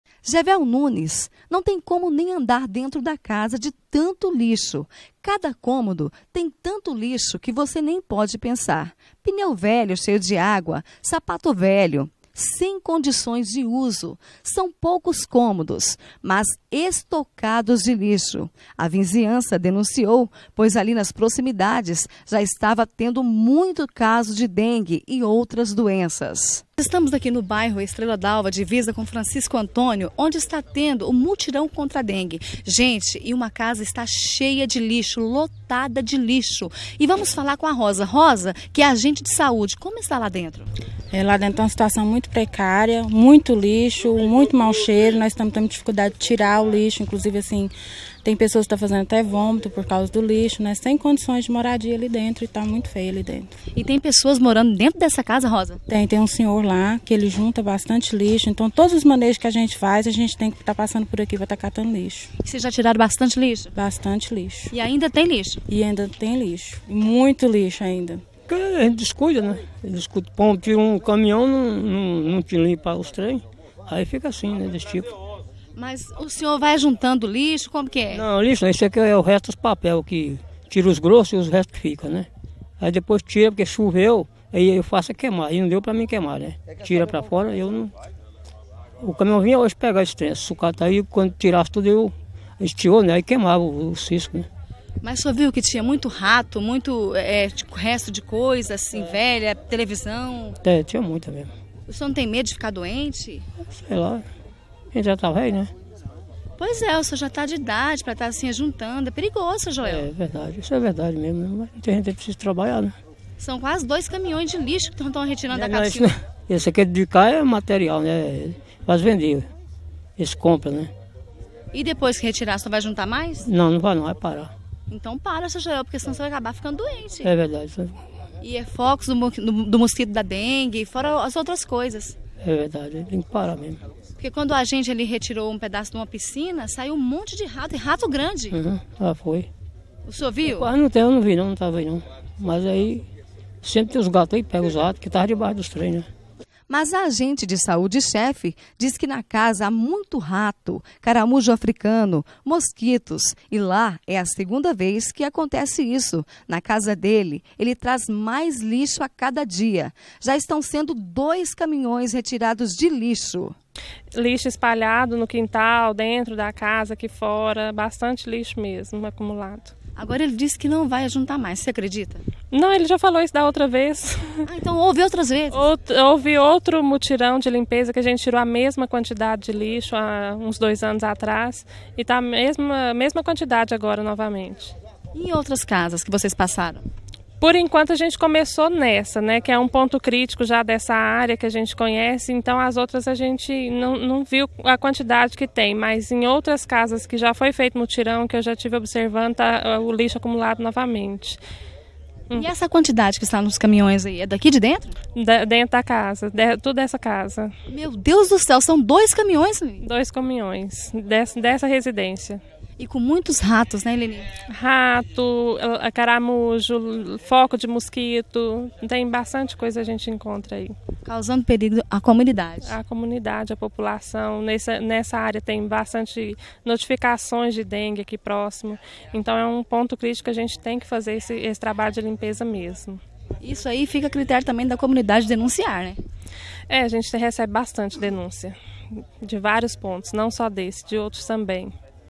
Mutirão da Dengue
Mais uma edição do Mutirão da Dengue, acontece em Jataí. Casa no bairro Francisco Antônio é denunciada com muito lixo e foco da dengue.